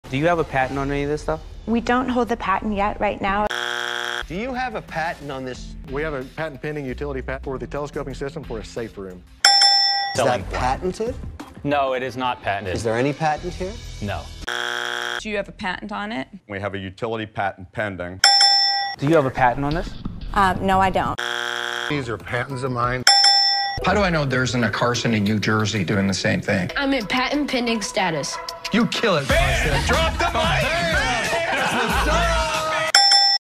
The Sharks Want to See a Patent
Swimming with the Sharks? Don't forget your life jacket! 🦈💡 Before diving into the tank, make sure your invention is protected with a patent 🤝